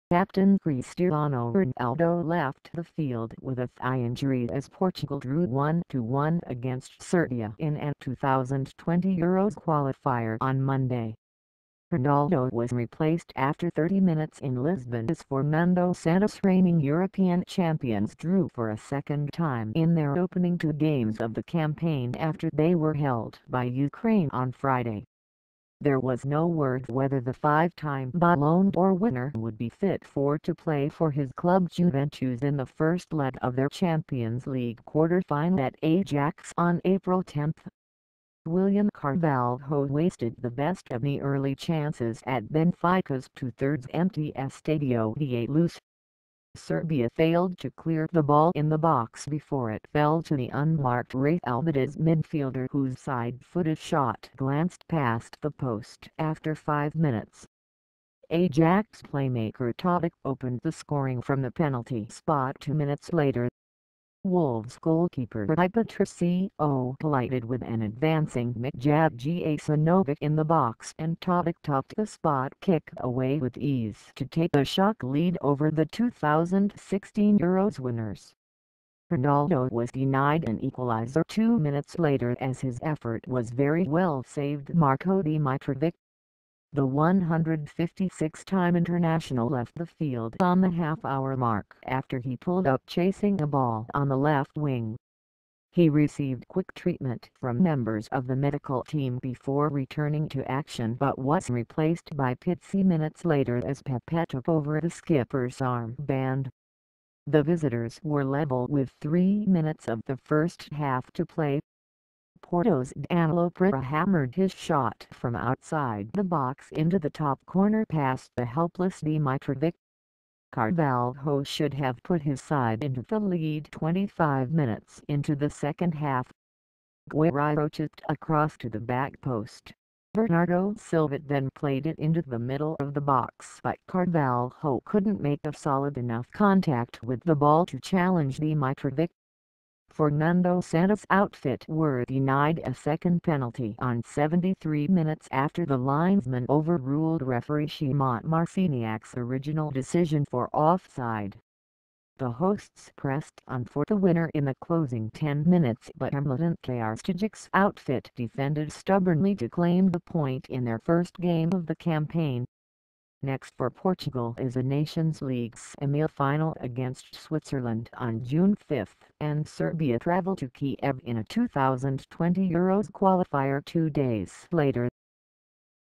Ronaldo injured as Portugal held by Serbia
Captain Cristiano Ronaldo left the field with a thigh injury as Portugal drew 1-1 against Serbia in an Euro 2020 qualifier on Monday.

Ronaldo was replaced after 30 minutes in Lisbon as Fernando Santos' reigning European champions drew for a second time in their opening two games of the campaign after they were held by Ukraine on Friday.

There was no word whether the five-time Ballon d'Or winner would be fit for to play for his club Juventus in the first leg of their Champions League quarter-final at Ajax on April 10.

William Carvalho wasted the best of the early chances at Benfica's two-thirds empty Estadio da Luz.

Serbia failed to clear the ball in the box before it fell to the unmarked Real Betis midfielder who's side-footed shot glanced past the post after five minutes.

Ajax playmaker Tadic opened the scoring from the penalty spot two minutes later.

Wolves goalkeeper Rui Patricio collided with an advancing Mijat Gacinovic in the box and Tadic tucked the spot kick away with ease to take a shock lead over the Euro 2016 winners.

Ronaldo was denied an equaliser two minutes later as his effort was very well saved Marko Dmitrovic.